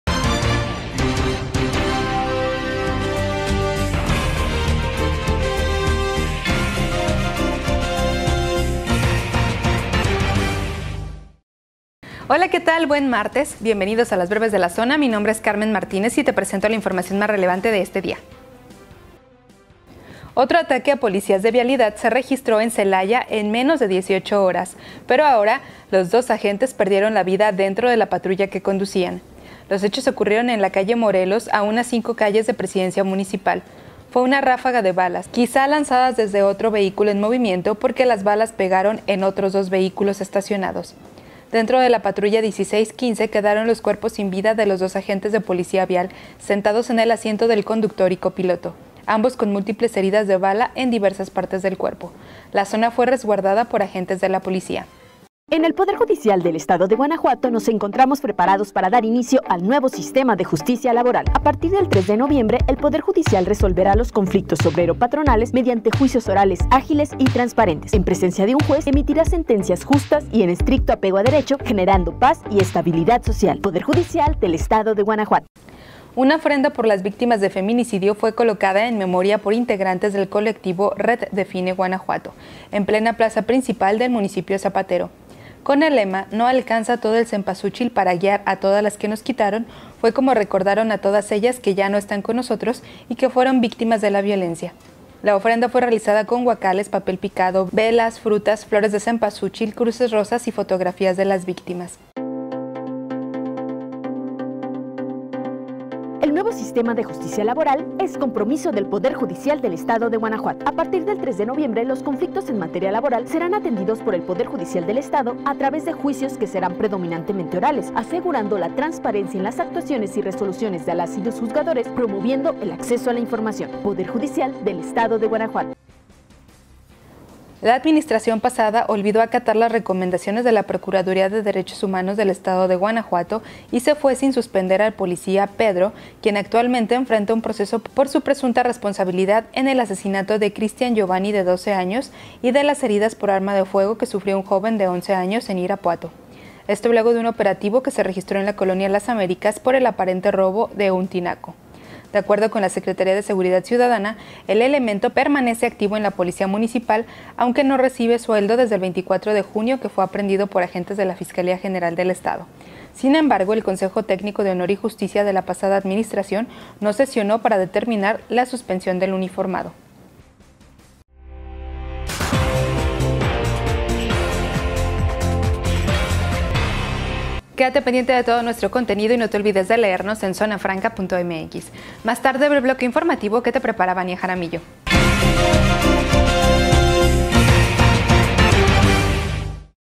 Ejecutan a dos policías de vialidad en Celaya | BrevesDeLaZona📰
Estas son las ÚltimasNoticias
- Con ofrenda recuerdan a mujeres víctimas de feminicidio en León.
- Sigue activo policía acusado de asesinar a un menor en Irapuato.
- Ejecutan a dos policías de vialidad en Celaya.